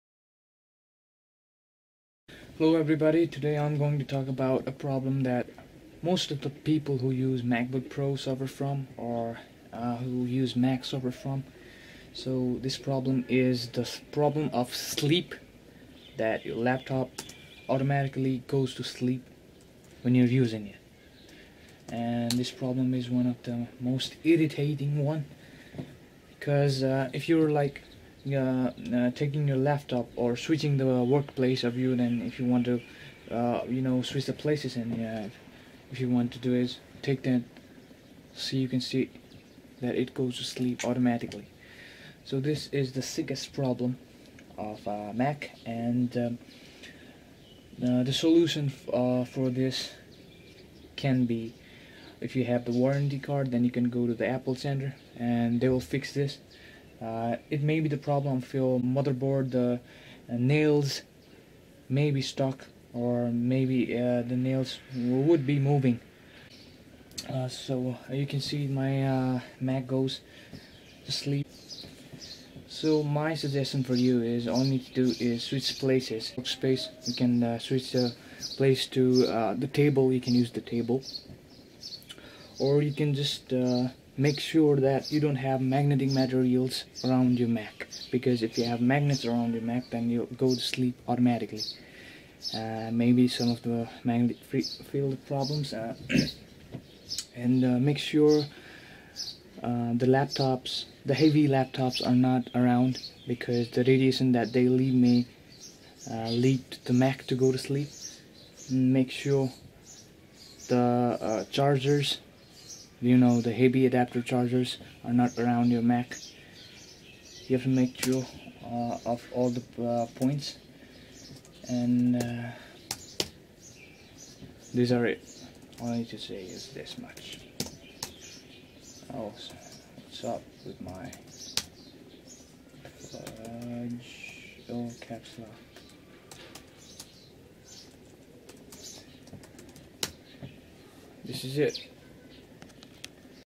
HOW TO FIX SLEEP PROBLEM IN MACBOOKS/RANDOM SLEEP
FIX THE SLEEP PROBLEM IN MAC IN A MINUTE. GET SOME ADDITIONAL INFORMATION REGARDING THE SLEEP PROBLEM AND SOME PREVENTIONS.
FIX THE PROBLEM WITHIN SOME MINUTES